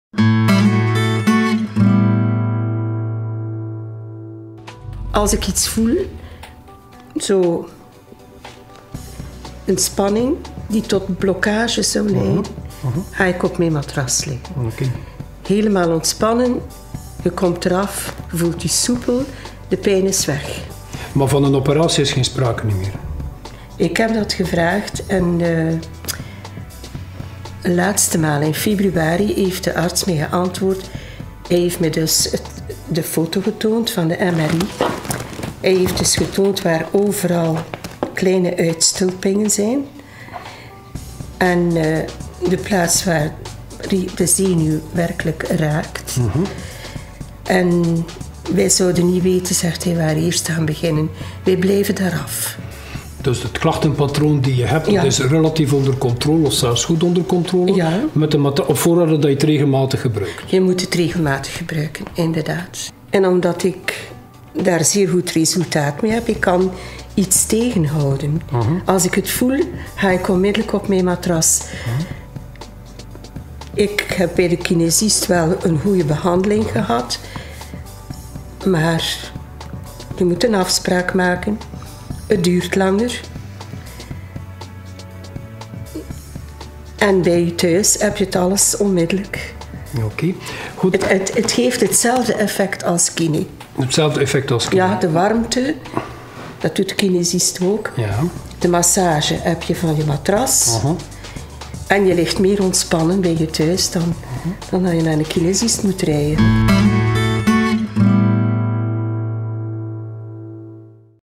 Rugspecialist Guy Declerck interviewt patiënt - andullatie therapie
Rugpijn, hernia, artrose, osteoporose, rugklachten, fibromyalgie, discushernia, ischias, nekpijn, hoofdpijn.  Allemaal aandoeningen die men met andullatie kan verbeteren.